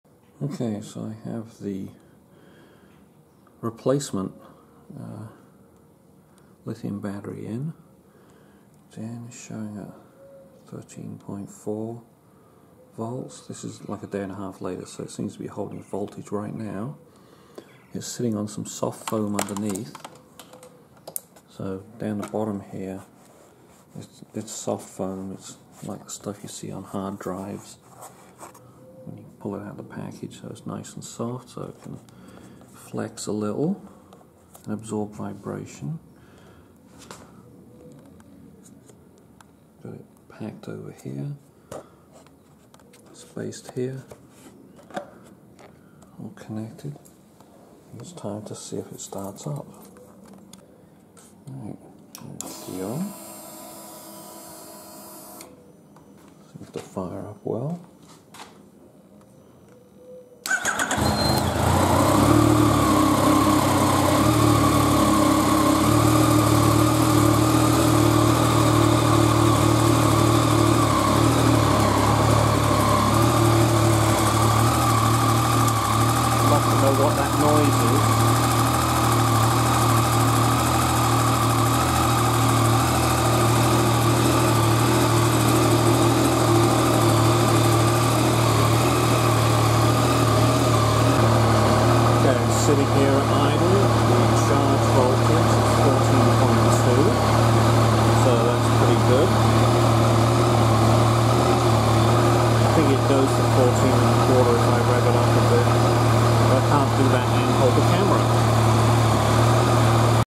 New Battery FZ 09
Replacing the Lead acid battery with a Lithium Iron Phosphate battery from battery tender. First LiFePO4 didn't hold a charge after 4 weeks, lets see if this one can. The test will come in about a month when we shall see if it can fire up the engine just as fast as it is doing in this video